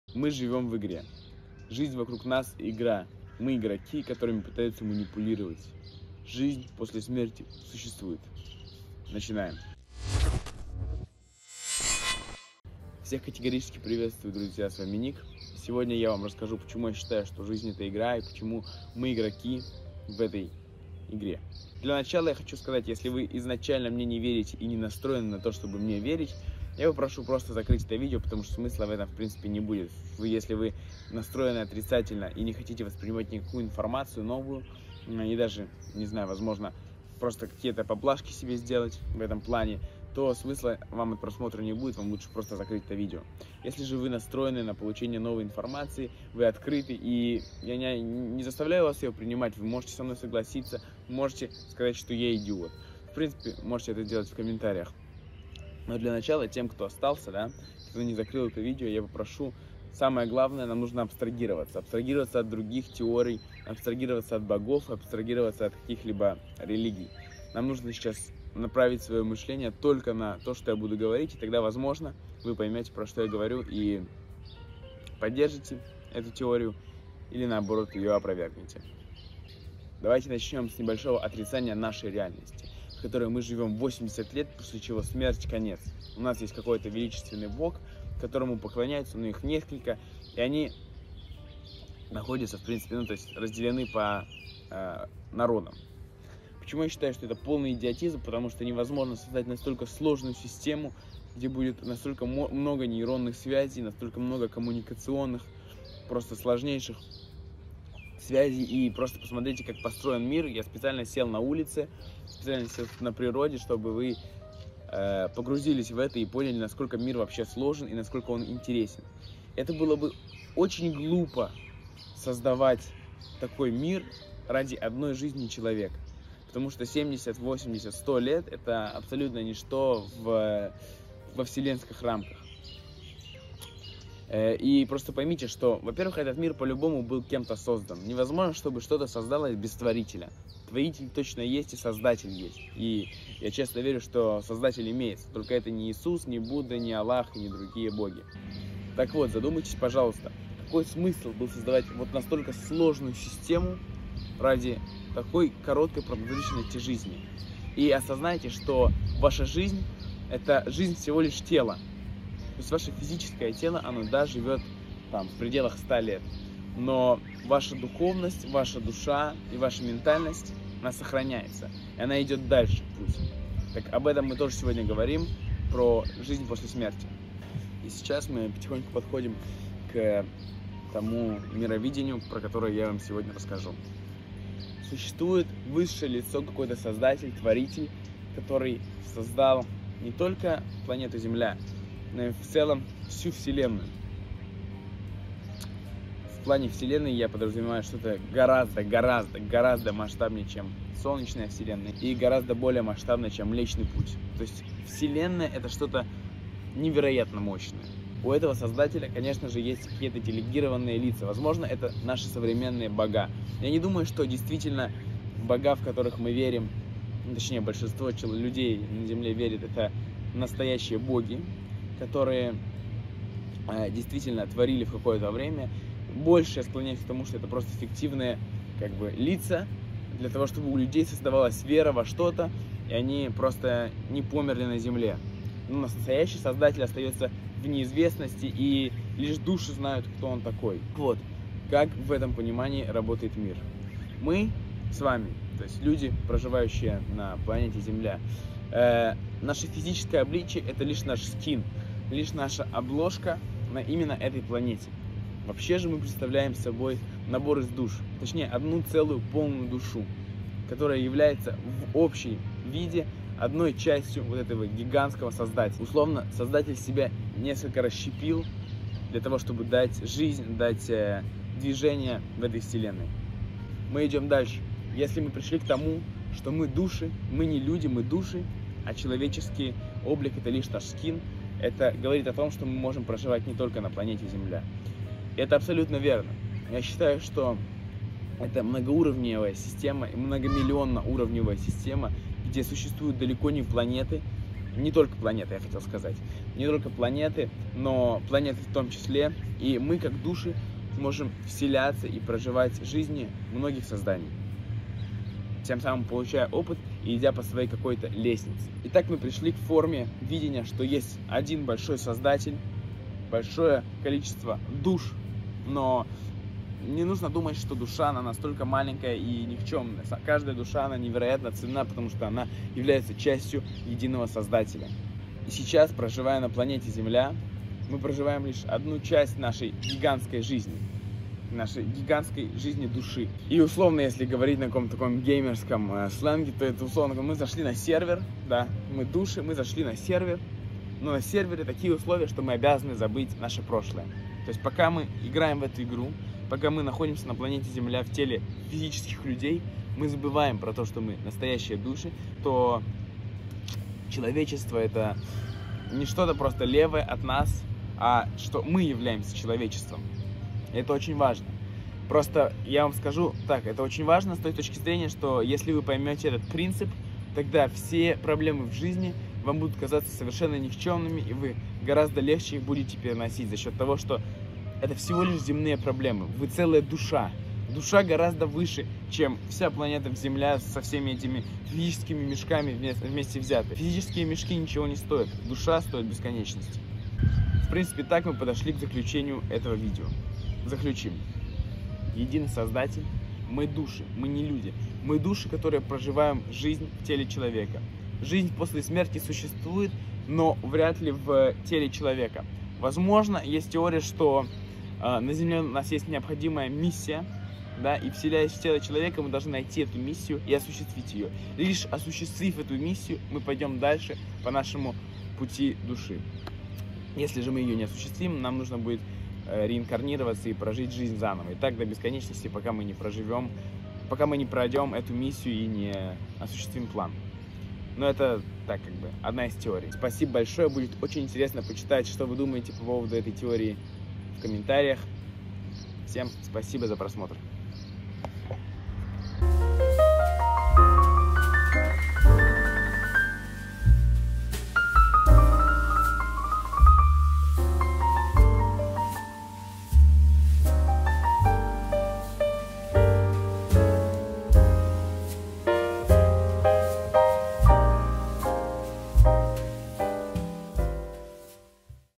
ТВОЯ ЖИЗНЬ-ИГРА. ТЕОРИЯ ДУШ
Твоя ЖИЗНЬ это ИГРА. ЖИЗНЬ после СМЕРТИ СУЩЕСТВУЕТ. Бог един и он создал эту вселенную.
Сегодня я расскажу вам интересную теорию устройства мира, которую сам считаю максимально похожей на реальную.